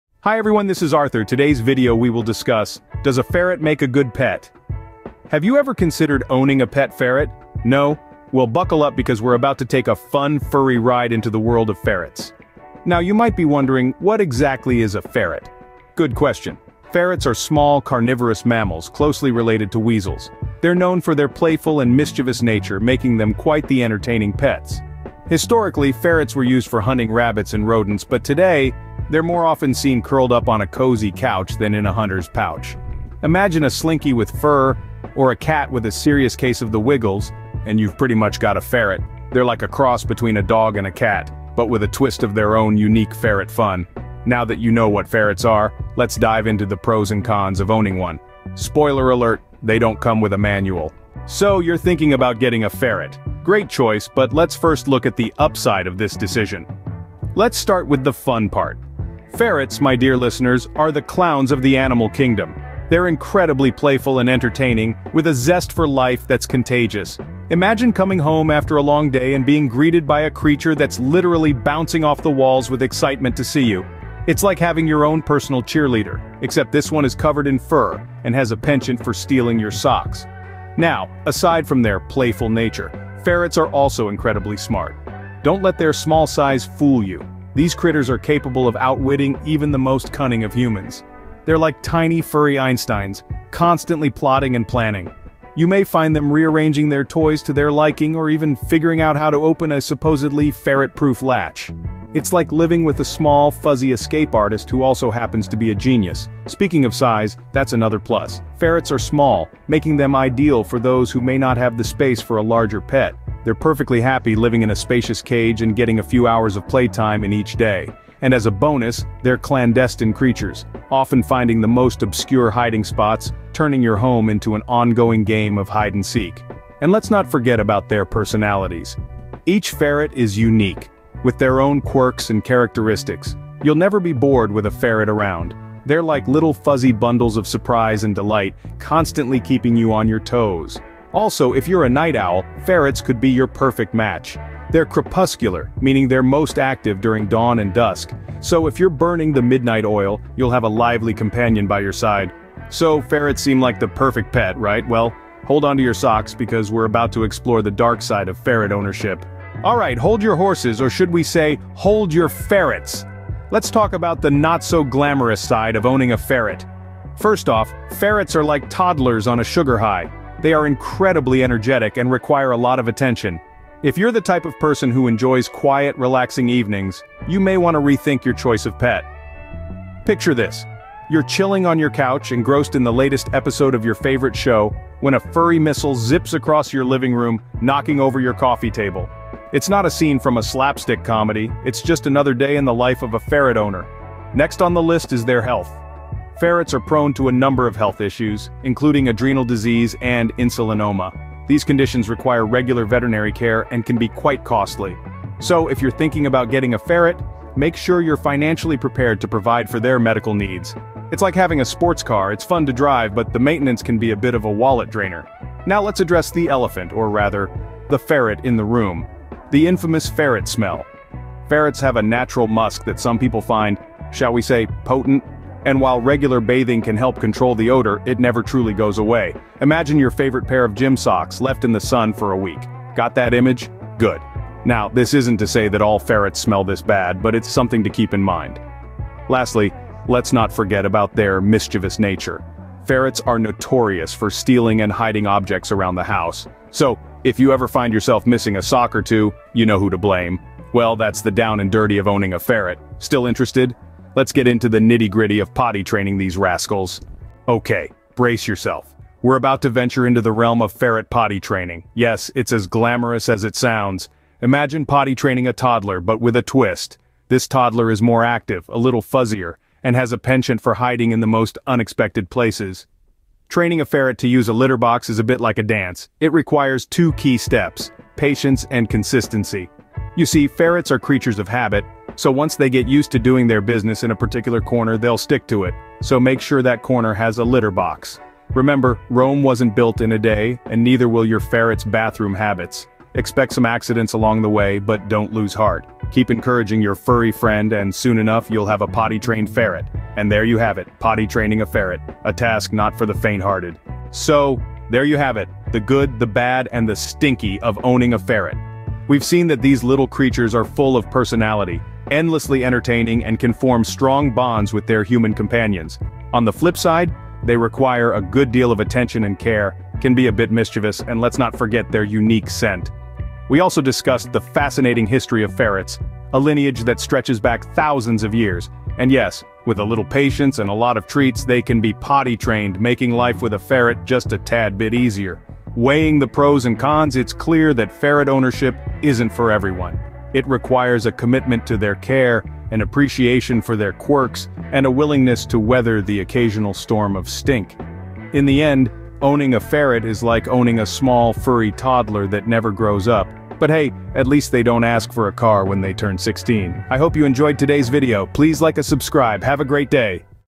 Does a ferret make a good pet?
In this video, I will discuss in this video "does a ferret make a good pet"? From uncovering their intriguing history to talking about the art of potty training, I will delve into the ins and outs of owning these adorable furry companions. Discover the pros and cons of welcoming a pet ferret into your home, all sprinkled with a touch of humor to keep you entertained throughout. If you're considering adding a furry friend to your family, this video is a must-watch! Dive in and let's explore the wonderful universe of pet ferrets together!

Arthur